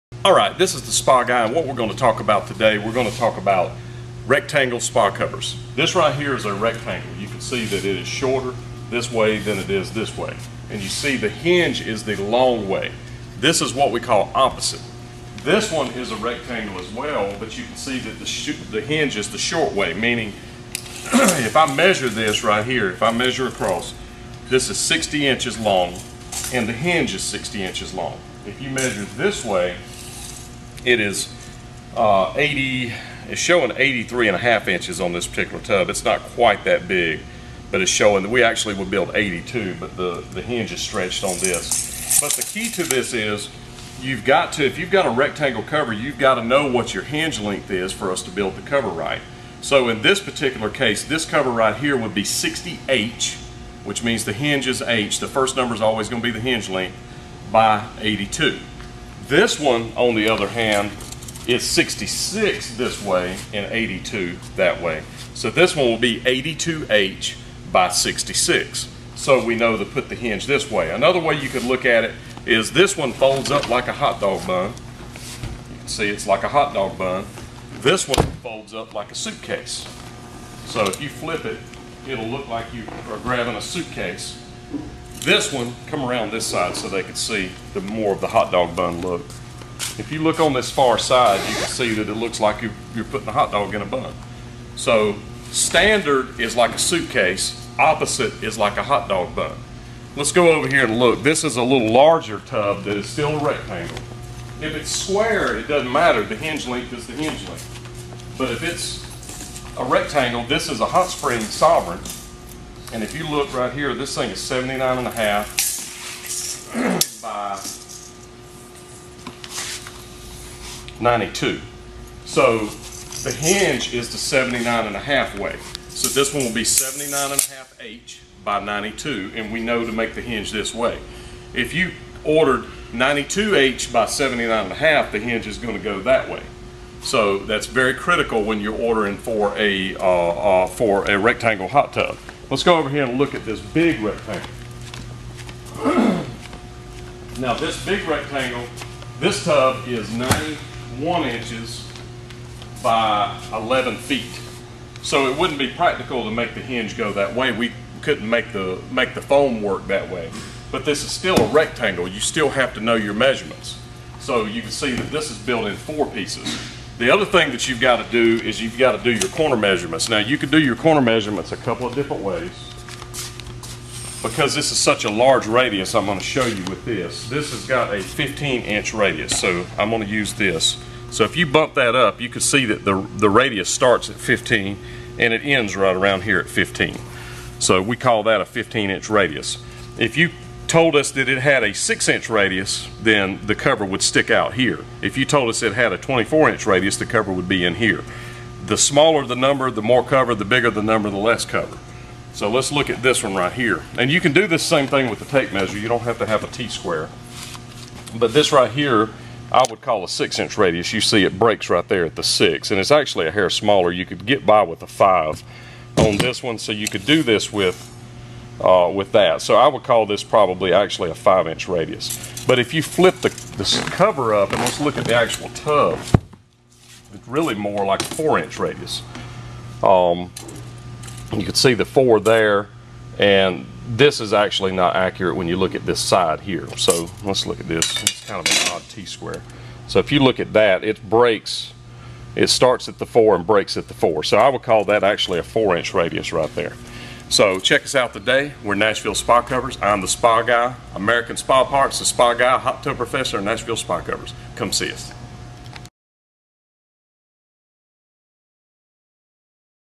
How To Measure Rectangle Shaped Spa Hot Tub Cover The Spa Guy Nashville Spa Covers
In this video The Spa Guy shows you how to measure a rectangle Spa Hot Tub Cover. He shows you the two different hinge styles and also how to measure the corner radius'. to purchase spa covers, Cover Cap Sunshields and Coverlifts.